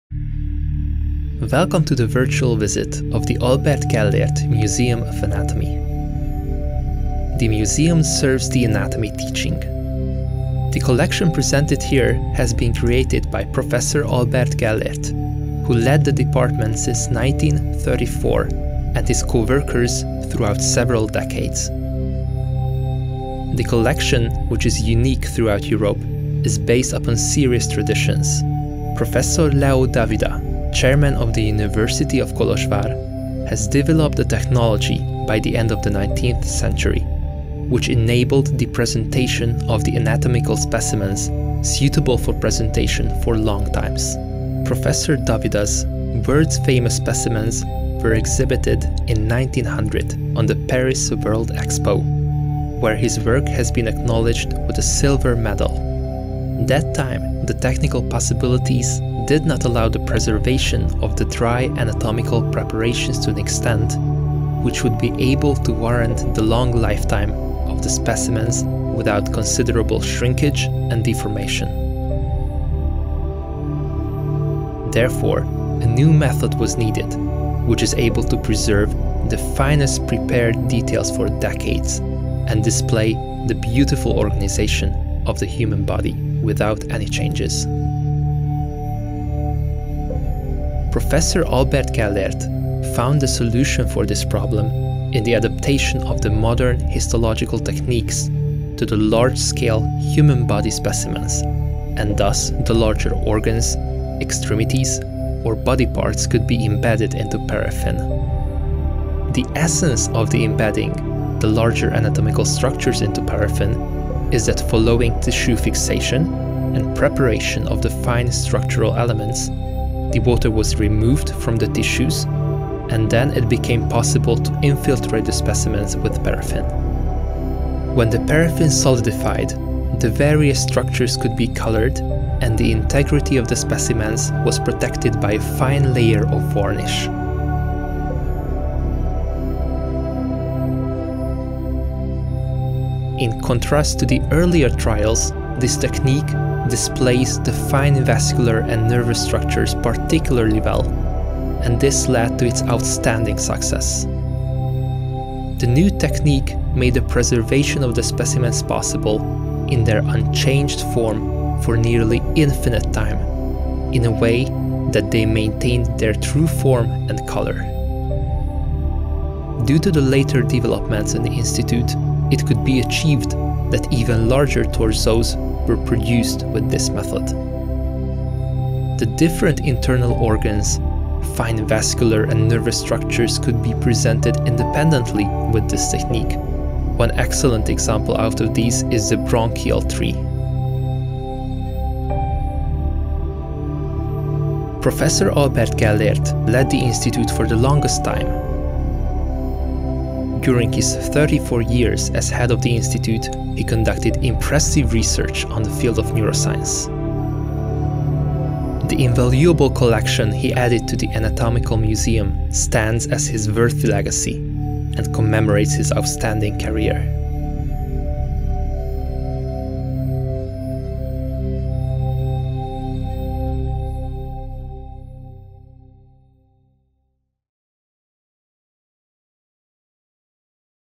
Guided tour of Anatomical Museum, University of Szeged, Albert Szent-Györgyi Medical School
Department of Anatomy, Histology and Embryology
Albert Gellért Museum, Szeged
Head of Department:
Prof. Antal Nógrádi MD., Ph.D., D.Sc.
Producer: Sándor Kecse-Nagy Head of Communication, SZTE SZAOK
Made by ZengoFilm:
Cameraman: Márk Valach